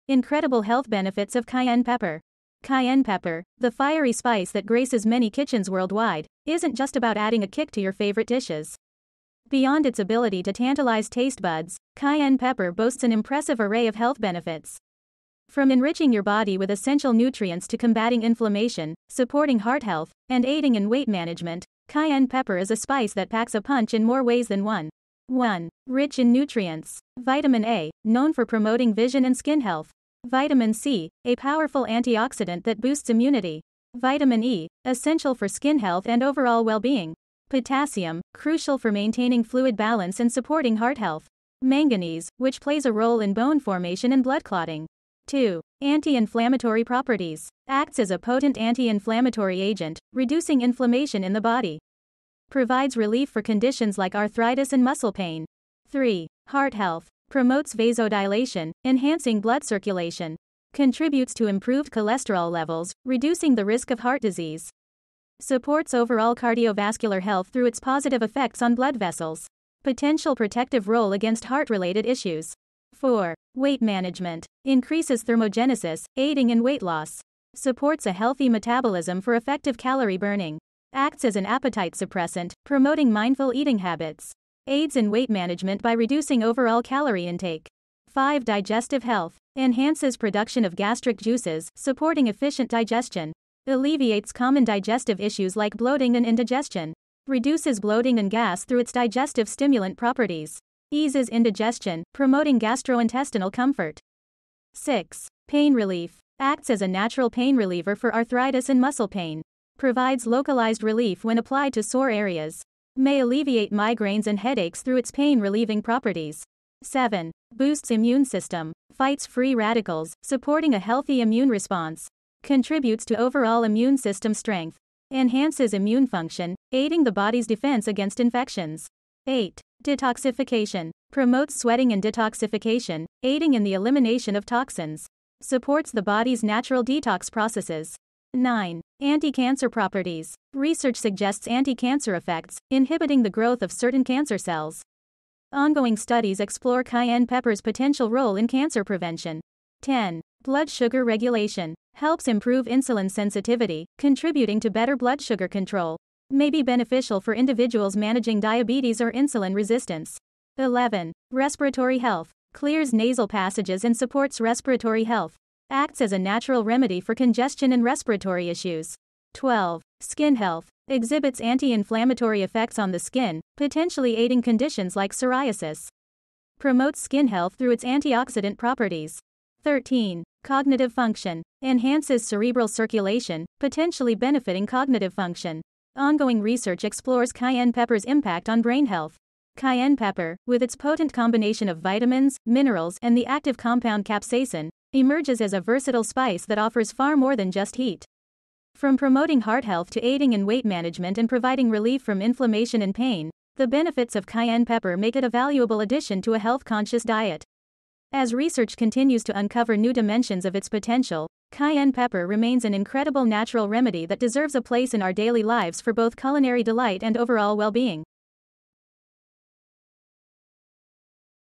Incredible Health Benefits of Cayenne Pepper
Discover the astonishing advantages of cayenne pepper! This fiery spice boasts numerous health merits. Firstly, it revs up metabolism, aiding weight loss. Additionally, cayenne peppers contain capsaicin, a compound with potent pain-relieving properties. Moreover, this zesty ingredient enhances blood circulation, promoting heart health. Its anti-inflammatory qualities alleviate arthritis pain and soothe digestive troubles. Detoxifying effects are another perk, purifying the body. Plus, cayenne pepper curbs cravings and supports healthy digestion. Incorporate this vibrant spice into your diet for a plethora of unique well-being benefits!